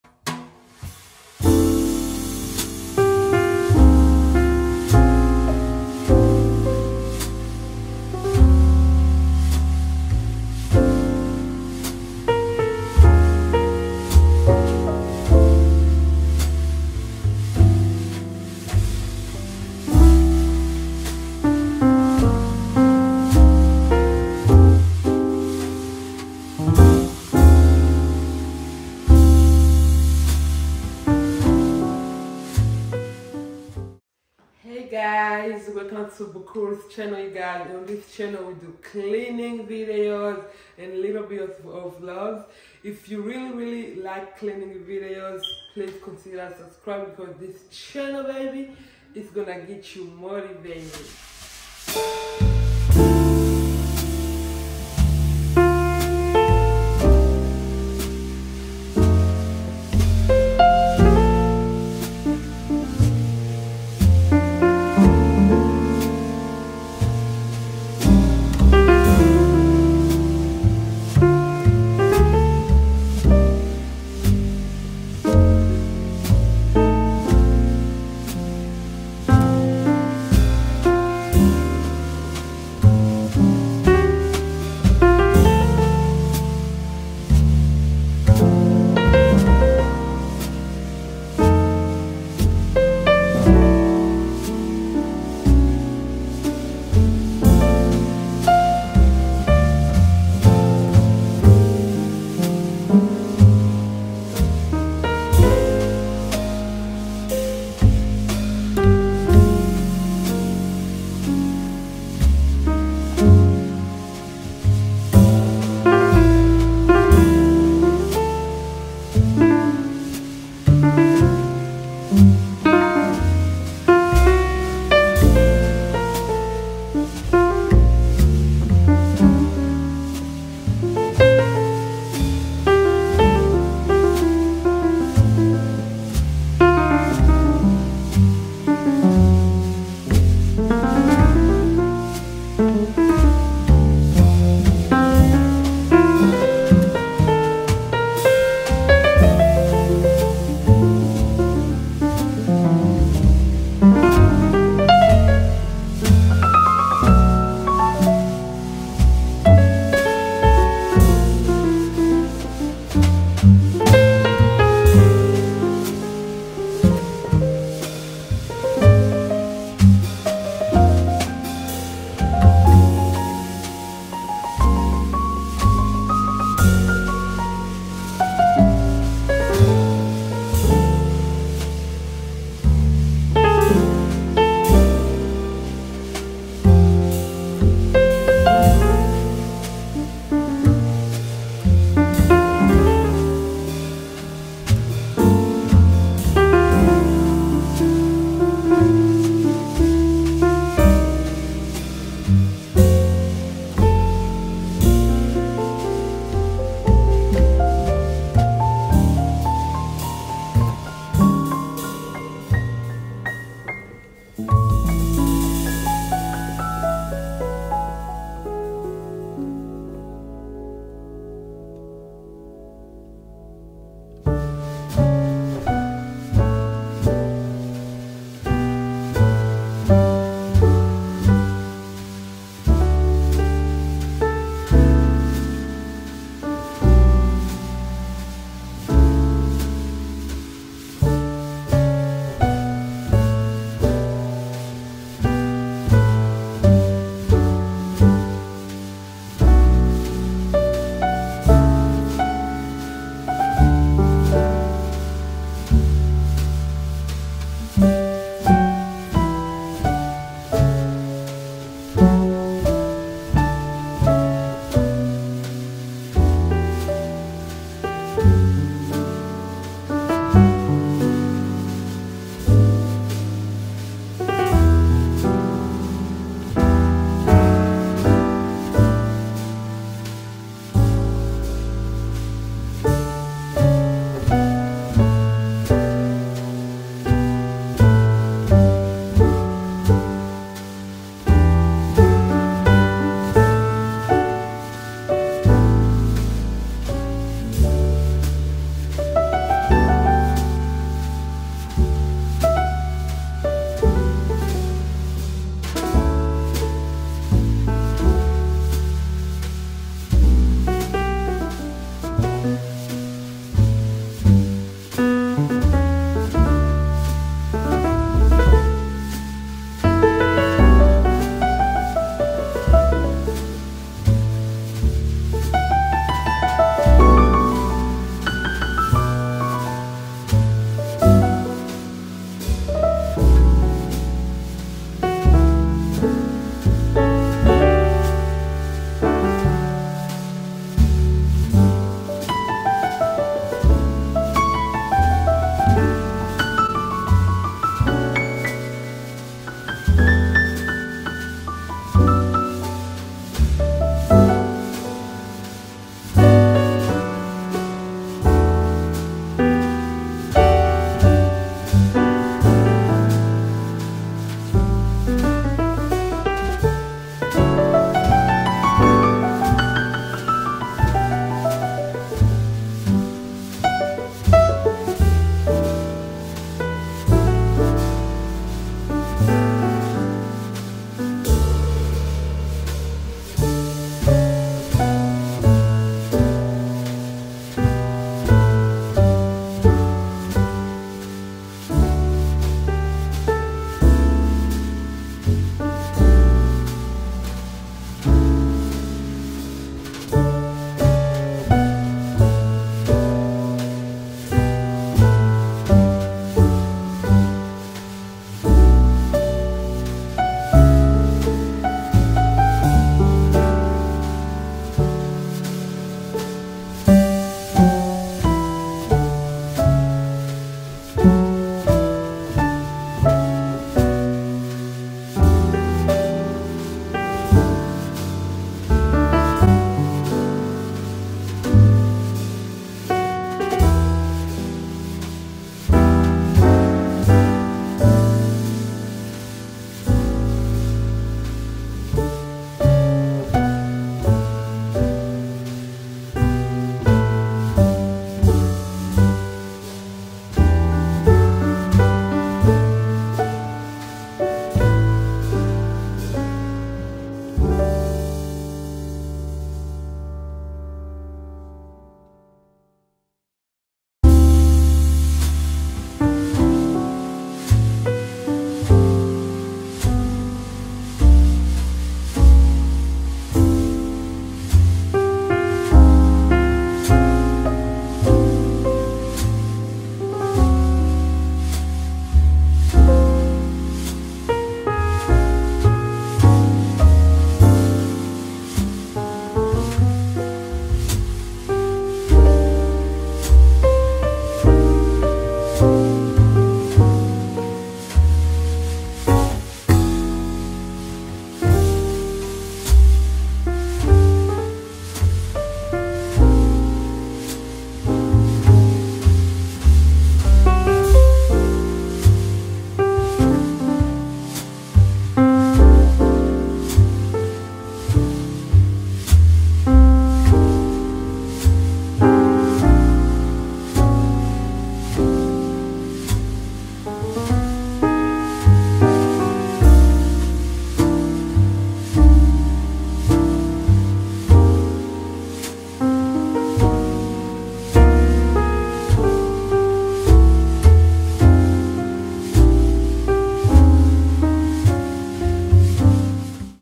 CLEAN WITH ME | PANTRY ORGANIZE WITH ME | CHILL VIBE
Hey lovers here i bring another cleaning video this video is chill vibe and i hope it puts you on the mood of cleaning as well.